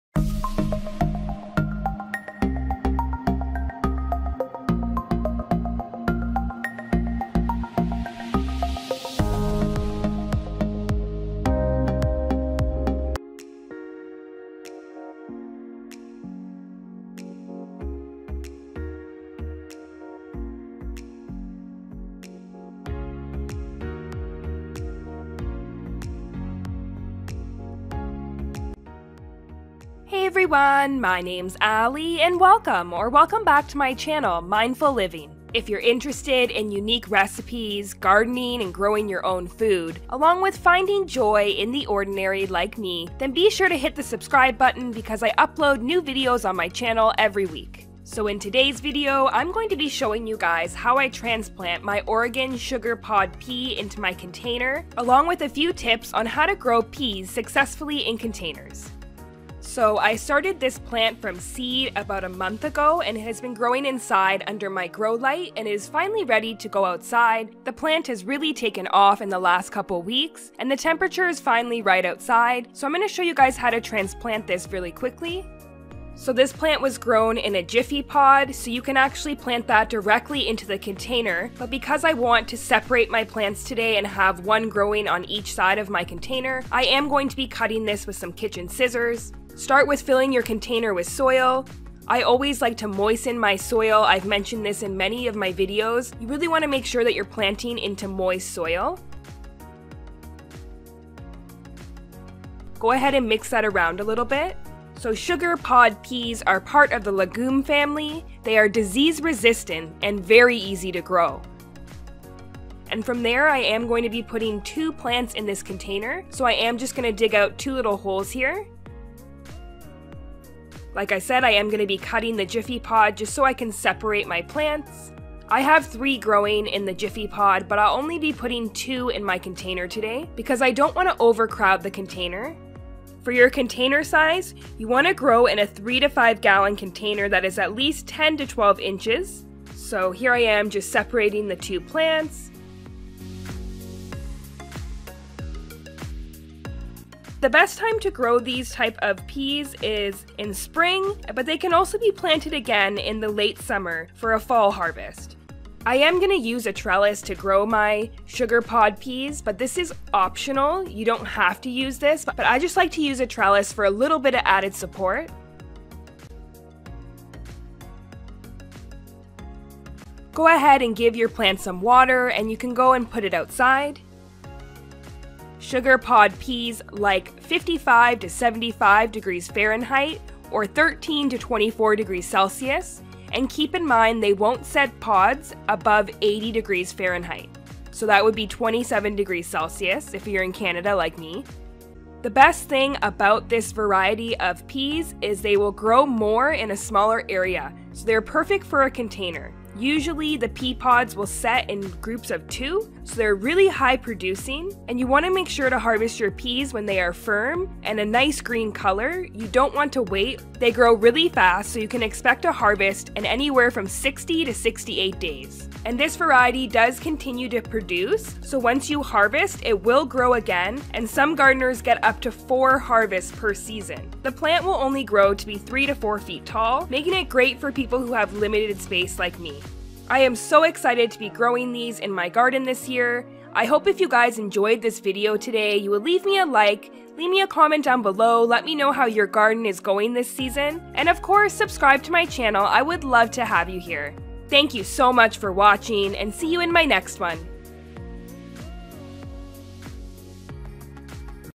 Growing Sugar Peas in Containers | Urban Balcony Garden
Sharing on my channel today some simple tips for growing Oregon sugar pod peas in containers! Follow along as I show you how I grew my sugar pod peas inside from seed, along with how to transplant and some tips on how to successfully grow sugar peas in containers. These peas are high producing, disease resistant and so easy to grow. They only grow to about 3-4ft making them perfect for growing in containers and small spaces.